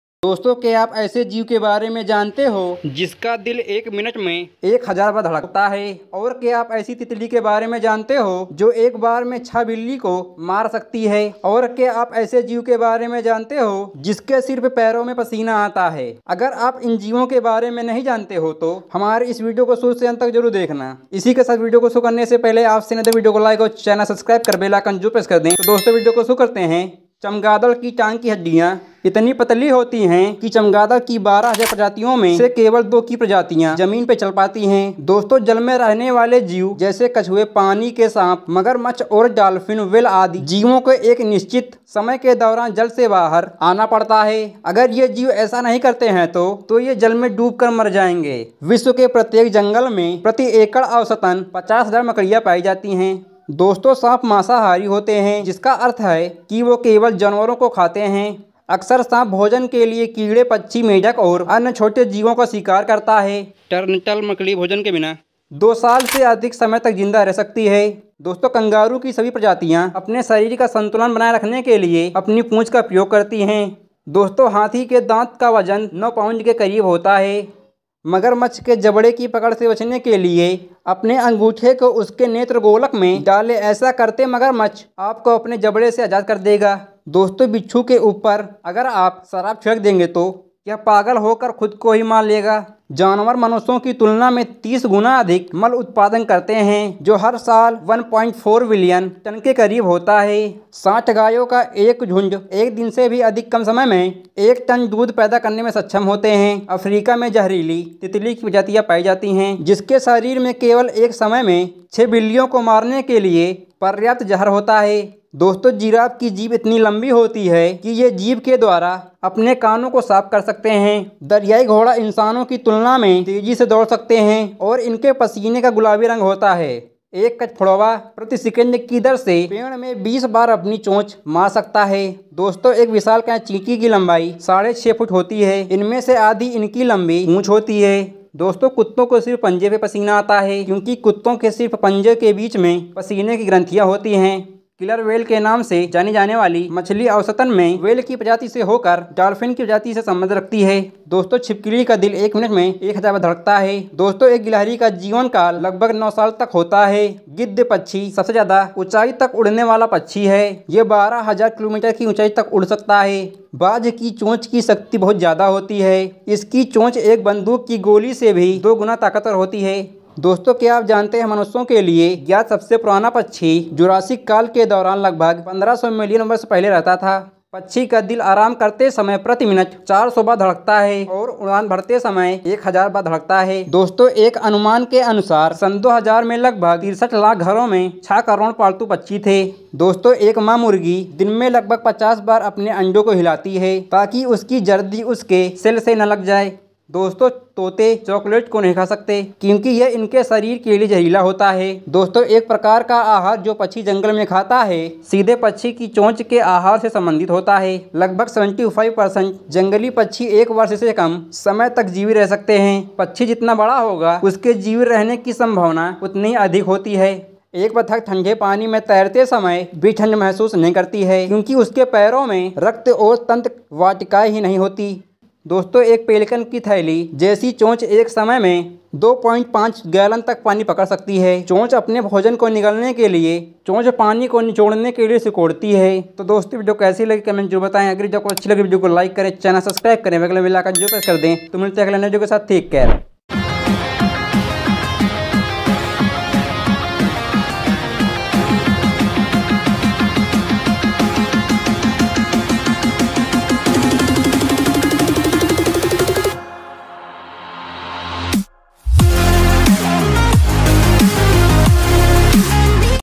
पानी में रहने के बावजूद भी मर जाते हैं ये जीव !! Board Classes
Title = पानी में रहने के बावजूद भी मर जाते हैं ये जीव !! Board Classes

Description in the Hindi = पानी में रहने के बावजूद भी मर जाते हैं ! ये जीव ! दोस्तों के आप ऐसे जीव के बारे में जानते हैं ! इसका दिल 1 मिनट में 1000 बार धड़कता है ! और क्या आप ऐसी तितली के बारे में जानते हैं  ! जो एक बार में अच्छा बिल्ली को जान से मार सकती है ! और क्या पैसे जू के बारे में जानते हैं ! जिसके सिर्फ पैरों  में पसीना आता है ! जंगली जानवरों के बारे में ऐसी बातें जो आप शायद ही जानते होंगे ! पक्षियों के बारे में अनोखी बातें !

Description in the English = They die even after living in water! This creature! Friends, do you know about such creatures! Its heart beats 1000 times in 1 minute. And do you know about such a butterfly! Which can kill a good cat in one go! And what do you know about money zoo! Whose feet only sweat! Such things about wild animals that you would hardly know! Amazing facts about birds!